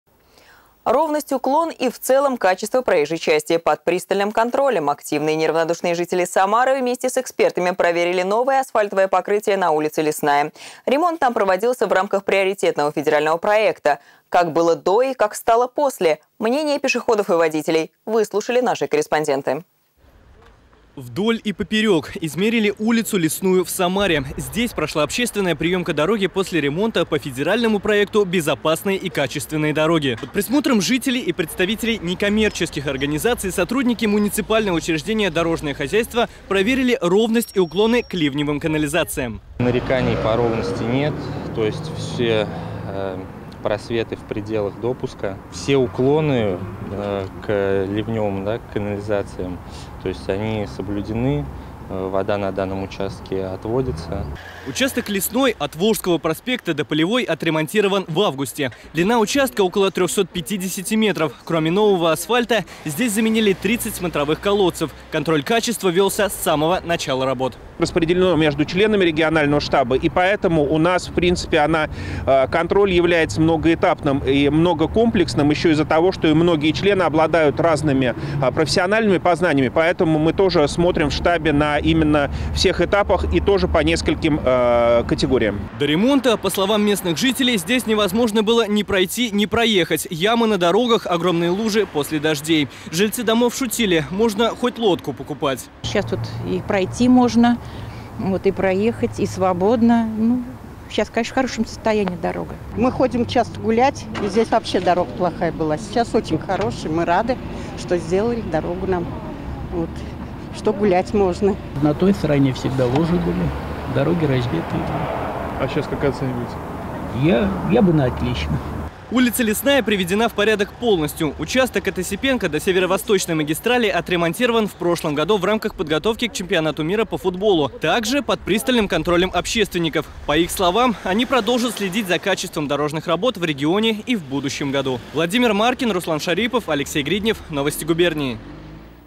Общественники и эксперты оценили новое асфальтовое покрытие на улице Лесной в Самаре
Ровность, уклон и качество проезжей части - под пристальным контролем. Активные и неравнодушные жители Самары вместе с экспертами проверили новое асфальтовое покрытие на улице Лесная. Ремонт там проводился в рамках приоритетного федерального проекта. Как было до и как стало после? Мнение пешеходов и водителей выслушали наши корреспонденты.